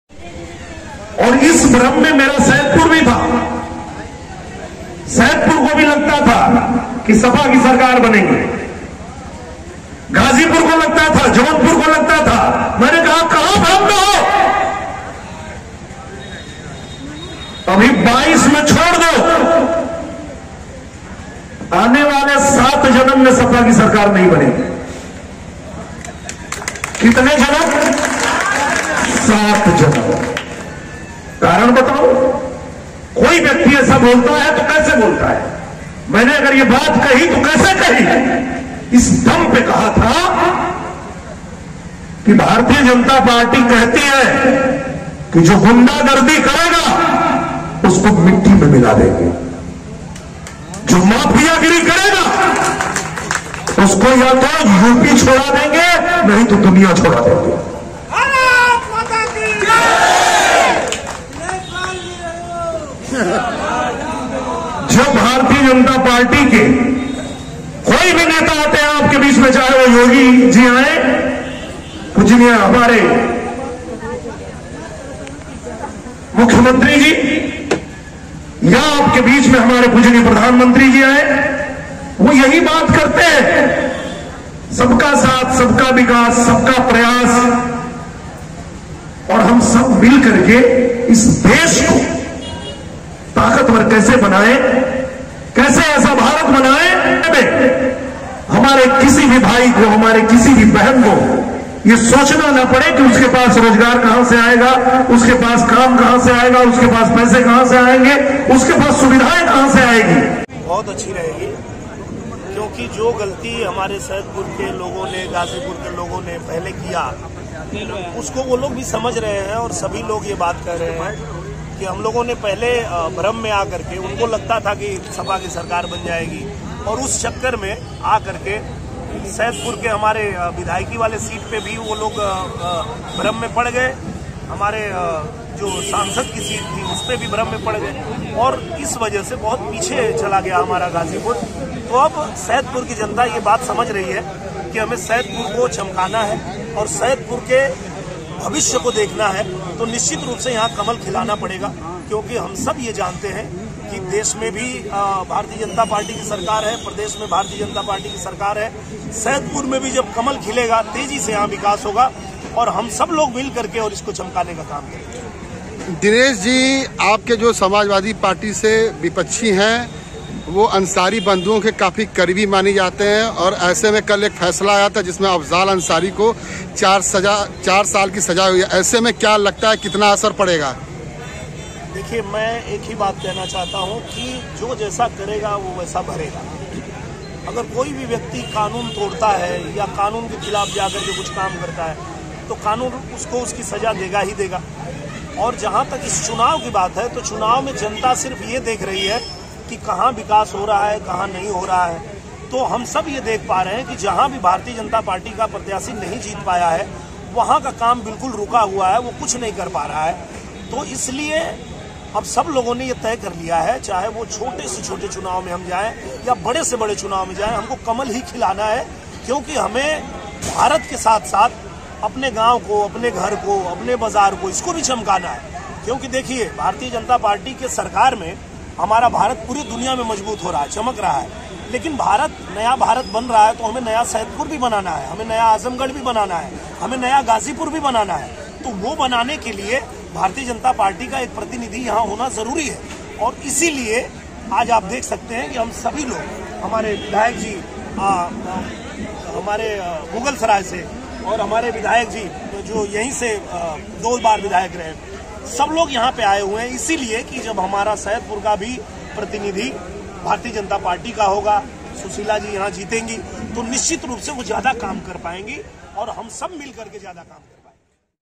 वाकई में!सात जन्म तक नहीं बनेगी समाजवादी पार्टी की सरकार#ghazipur#bjp#sp#akhileshyadav
MMC NEWS
नगर निकाय चुनाव के प्रथम चरण में गाजीपुर जनपद में 4 मई को मतदान होना है ऐसे में सभी राजनीतिक दलों ने अपने प्रचार प्रसार जोरों शोर पर कर दिया है इसी कड़ी में भारतीय जनता पार्टी ने भी अपने स्टार प्रचारक चुनावी मैदान में उतार दिए हैं गाजीपुर के ही रहने वाले और आजमगढ़ के सांसद दिनेश लाल यादव उर्फ निरहुआ ने बीती रात नगर पंचायत सैदपुर की प्रत्याशी सुशीला सोनकर के समर्थन में नुक्कड़ सभा में शामिल हुए और इस दौरान मंच से बोलते हुए उन्होंने कहा कि 2022 के चुनाव में सैदपुर के लोग भी भ्रम में थे कि समाजवादी पार्टी की सरकार बनेगी इसके अलावा गाजीपुर और जौनपुर के लोगों को भी लगता था लेकिन हम कहते हैं कि 2022 को छोड़ दो आने वाले सात जन्म में भी सपा की सरकार नहीं बनेगी इस दौरान उन्होंने योगी आदित्यनाथ के उस बयान को भी मंच से कहा कि जो गुंडागर्दी करेगा उसे मिट्टी में मिला देंगे और जो माफिया गिरी करेगा उसे यूपी छुड़ा देंगे या फिर दुनिया छुड़ा देंगे।

इस दौरान मीडिया से बातचीत करते हुए उन्होंने बताया कि गाजीपुर और सैदपुर के लोगों ने जो पिछले विधानसभा में गलती किया था अब वह लोग समझ रहे हैं वह लोग भ्रम में आ गए आ गए थे कि सपा की सरकार बनेगी और इसी भ्रम में सैदपुर विधानसभा और गाजीपुर लोकसभा सीट पर भी भ्रम में पड़कर गलती कर डाली। इस दौरान जब विपक्षी प्रत्याशी जो अंसारी बंधुओं के काफी करीब है उन पर सवाल के साथ ही अफजाल अंसारी की सजा पर उन्होंने कहा कि जो जैसा करेगा वैसा ही भरेगा जो व्यक्ति कानून के खिलाफ काम करेगा तो कानून उसे सजा जरूर देगी और चुनाव में जनता देख रही है कि कहां विकास हो रहा है और कहां पर विकास नहीं हो रहा है जहां प्रथम भारतीय जनता पार्टी का प्रत्याशी नहीं जीता है वहां पर बिल्कुल ही काम नहीं हो पा रहा है।

वहीं ओमप्रकाश राजभर के दिए गए बयान की सरकार जाति प्रमाण पत्र बनाना बंद कर दे तो जातिवाद बंद हो जाएगी इस पर उन्होंने कहा कि अगर उनका ऐसा मानना है तो लोग उस पर विचार करेंगे जो ज्यादा समझदार और ऊपर बैठे लोग हैं वह उस पर चिंतन करेंगे।